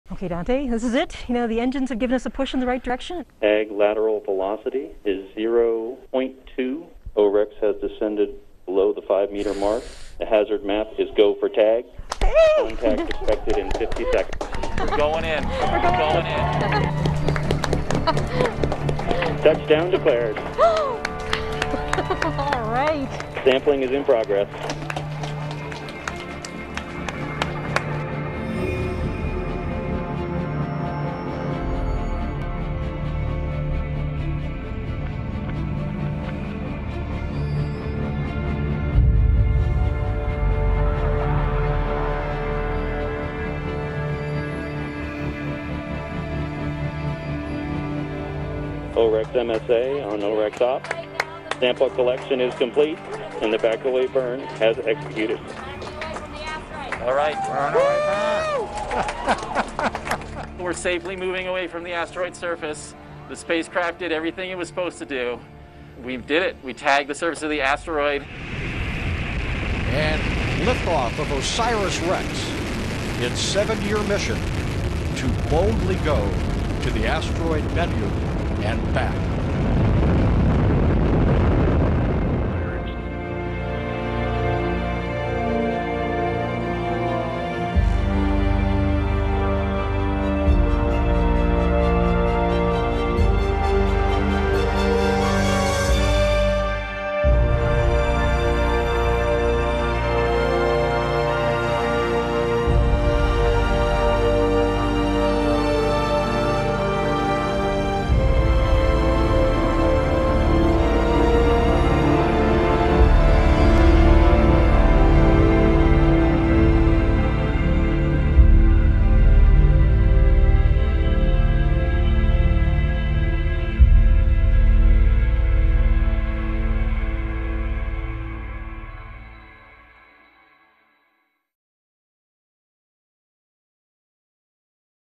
NASA probe “high-five” to asteroid - BBC URDU
ناسا کے زمین پر موجود سائنسدانوں نے لاکھوں کلومیٹر دور شہابیے کی سطح کو خلائی جہاز کے زریعے چند سیکنڈوں کے لیے چھوا۔ اس دوران خلائی جہاز اوسیرس ریکس نے شہابیے پر سے نمونے بھی جمع کیے۔ تفصیل اس ڈیجیٹل ویڈیو میں۔